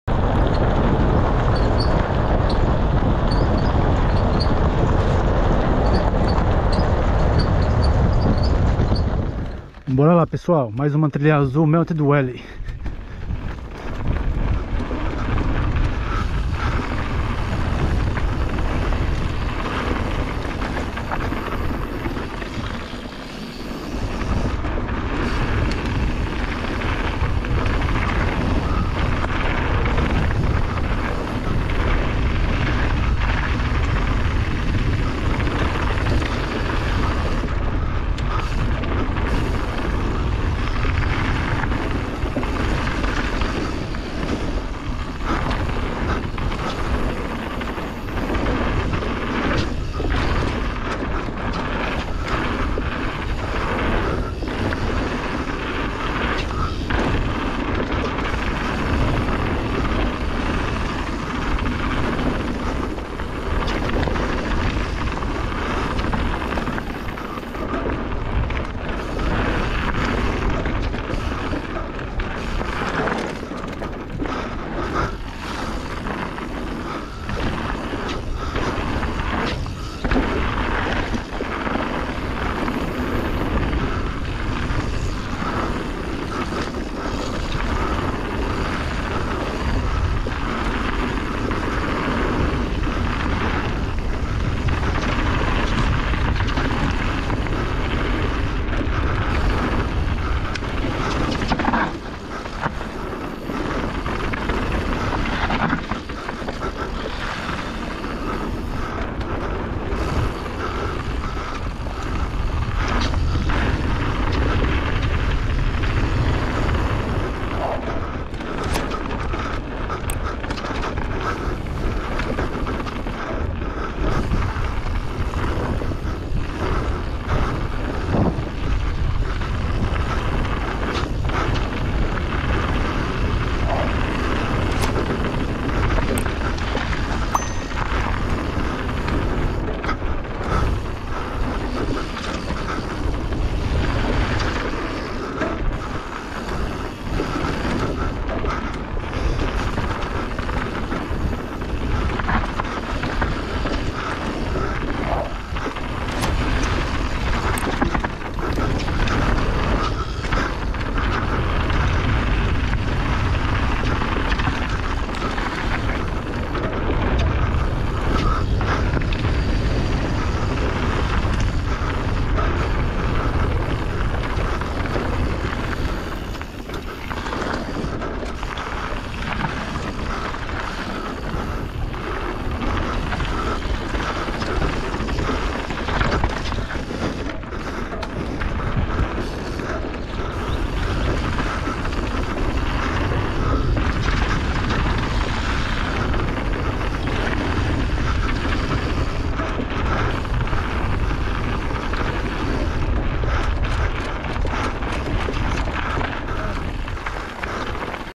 Pista de dh 1.8km ,bike park wales ,Melted Welly 2021
Mais uma trilha finalizada nesse lugar espetacular .